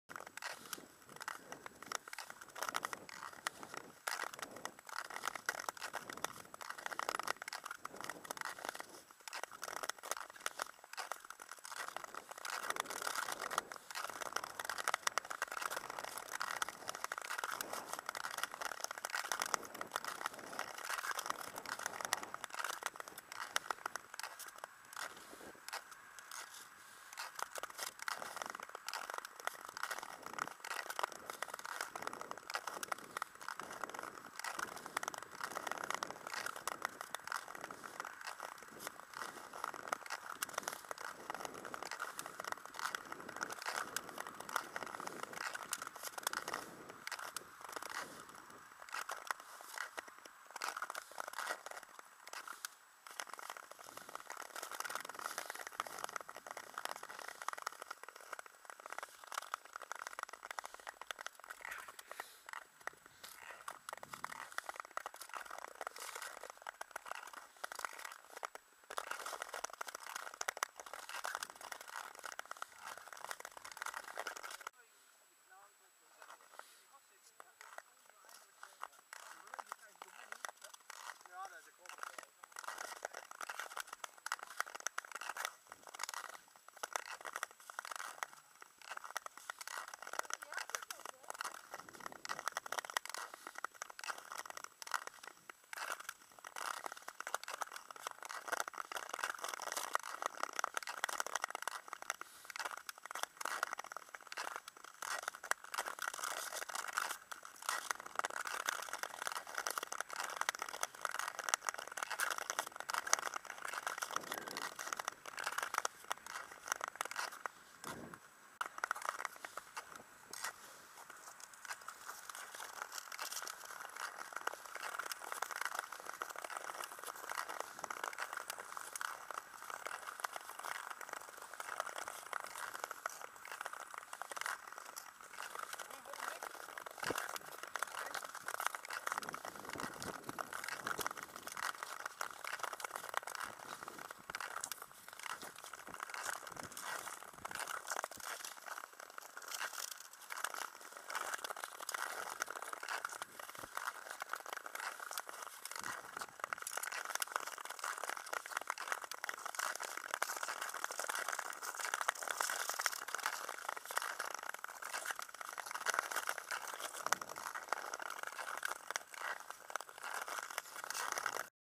Testronde ijsbaan Culemborg
Eerste ronde op de ijsbaan in Culemborg, 29 december 2010